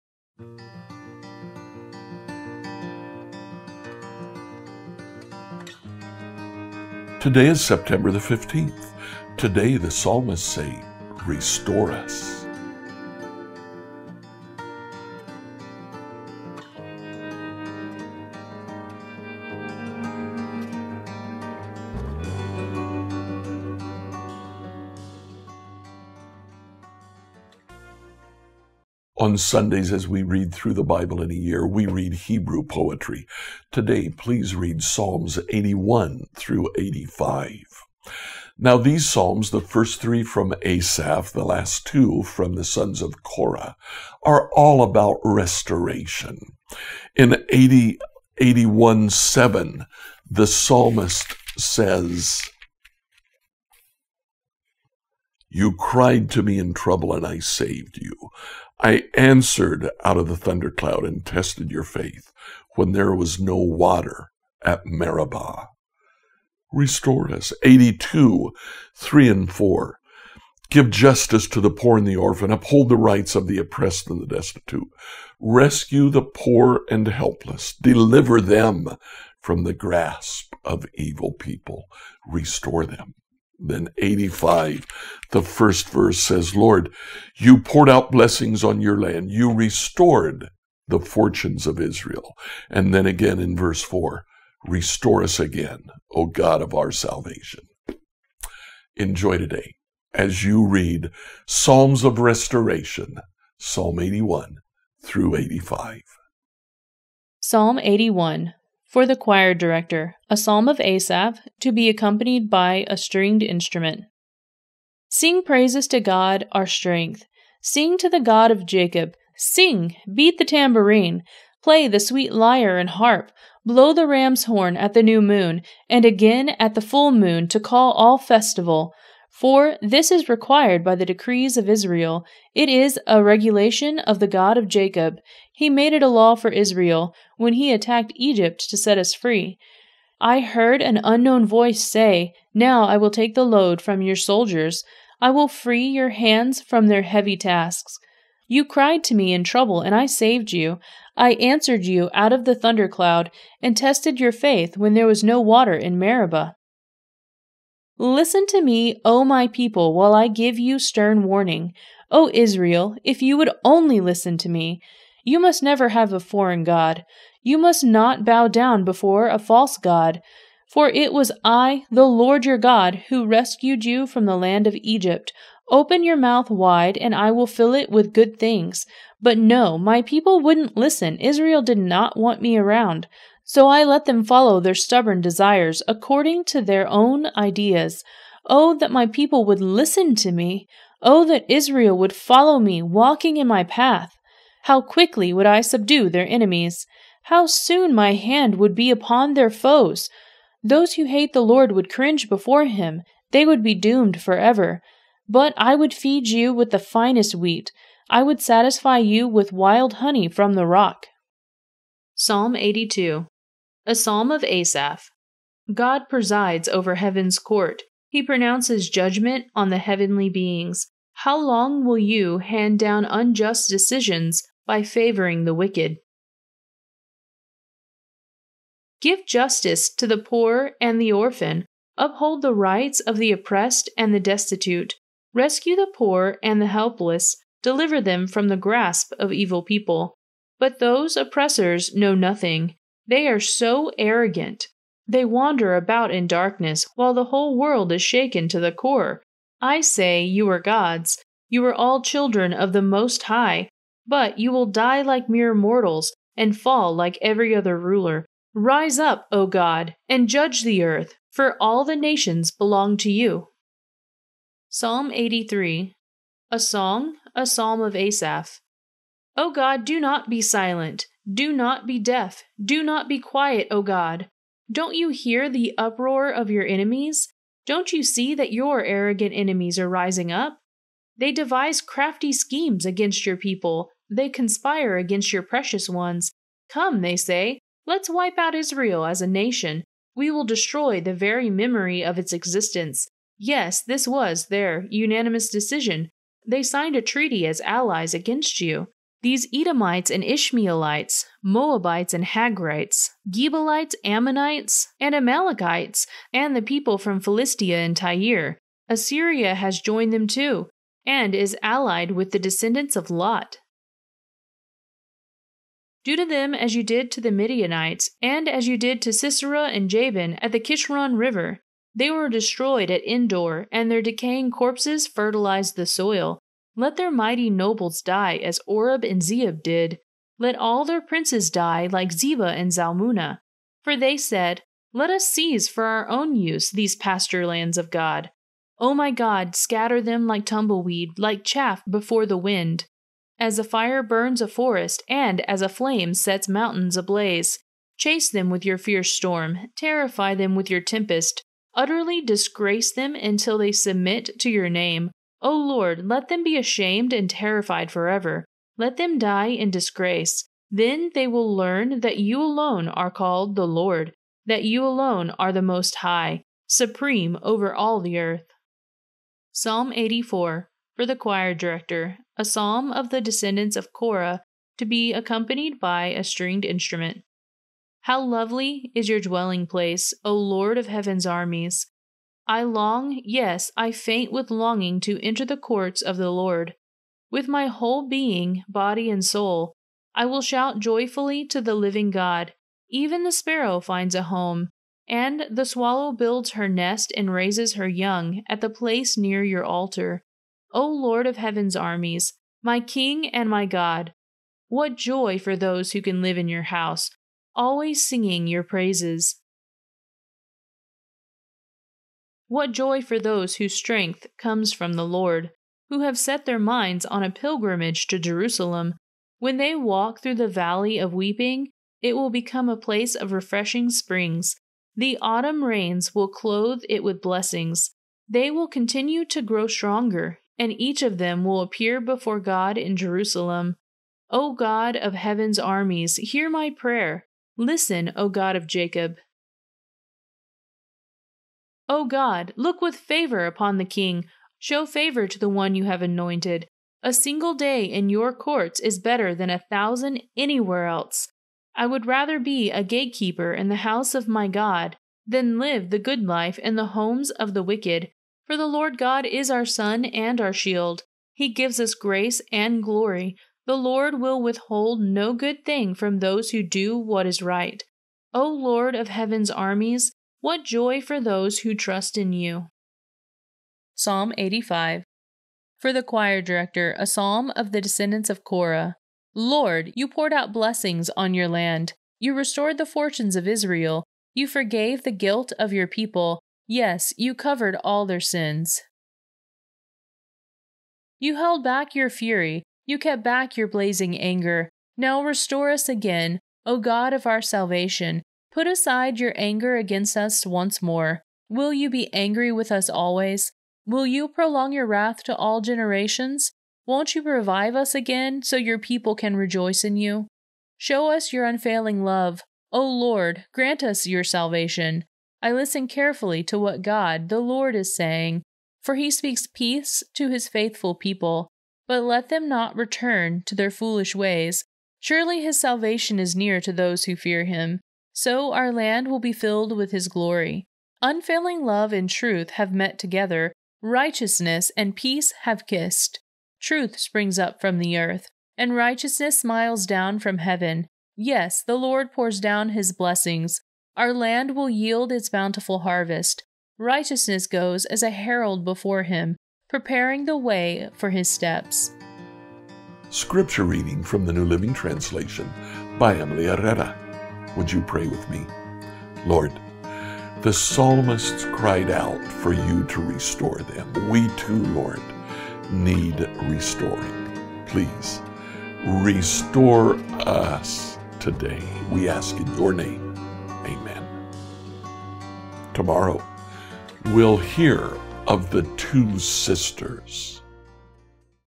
Psalm 81-85 | Restore us - September 15, 2024
In Psalms 81-85 the psalmists say to the Lord, “Restore us!” What did they mean? Let’s find out together as we read Psalms 81-85.